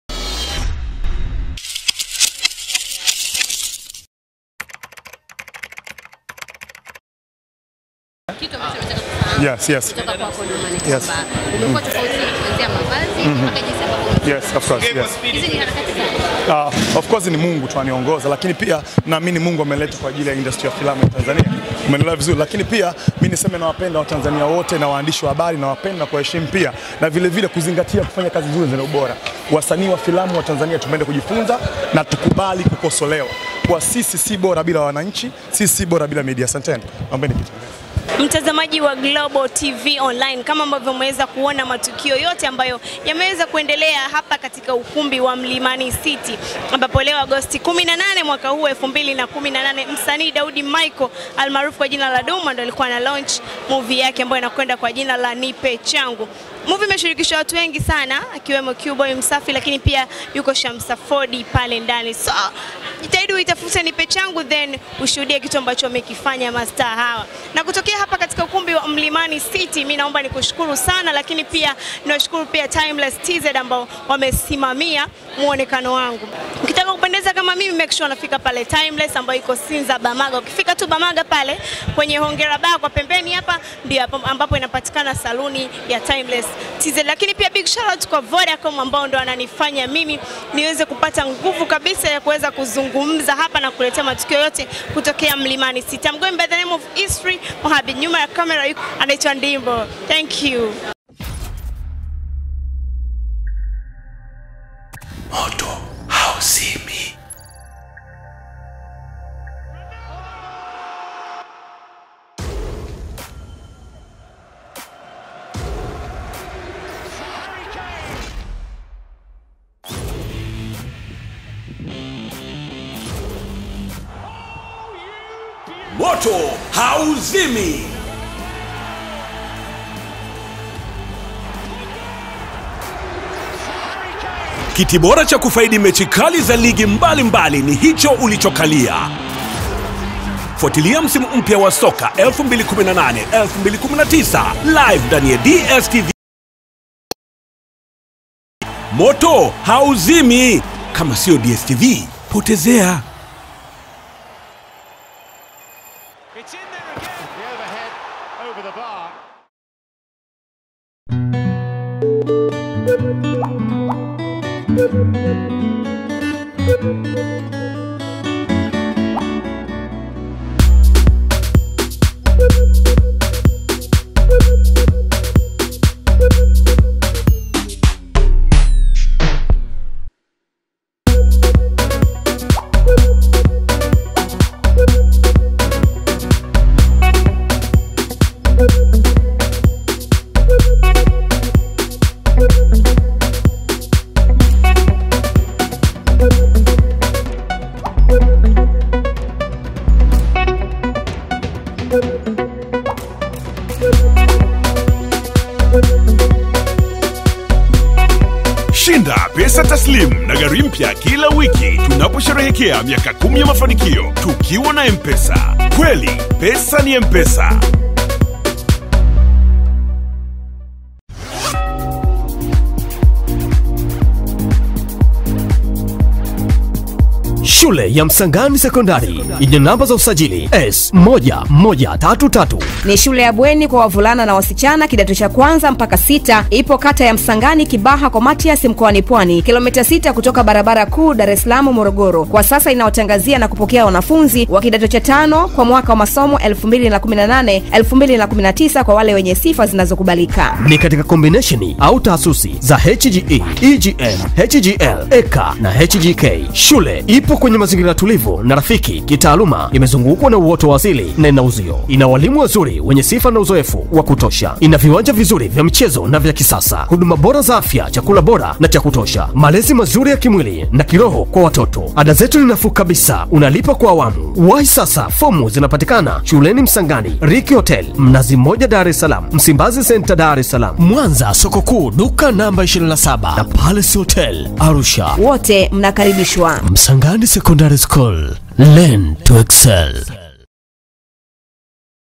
Alichokisema Duma Katika Uzinduzi Wa Filamu Yake Mpya
Msanii nguli wa Filamu nchini, Daudi Michael 'Duma' usiku wa Agosti 18 amefanya uzinduzi wa Filamu yake mpya inayoitwa Nipe Changu katika ukumbi wa Mlimani City jijini Dar.

Akizungumza na waandishi katika uzinduzi huo Duma amezishukuru media kwa kumsapoti na kutoa rai kwa wasanii wenziye kuwa waendelee kufanya kazi nzuri siku hadi siku.